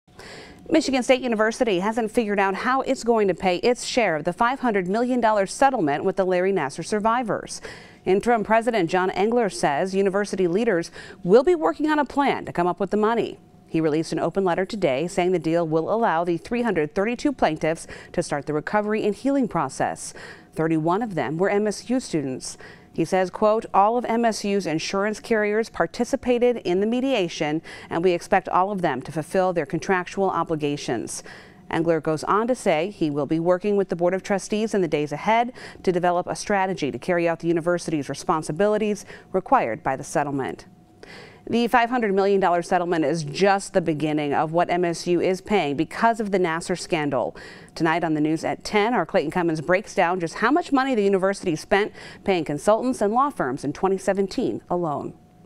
Engler releases letter after Nassar settlement
Michigan State University Interim President John Engler has sent a letter to the MSU community following a $500 million settlement with hundreds of survivors of Larry Nassar.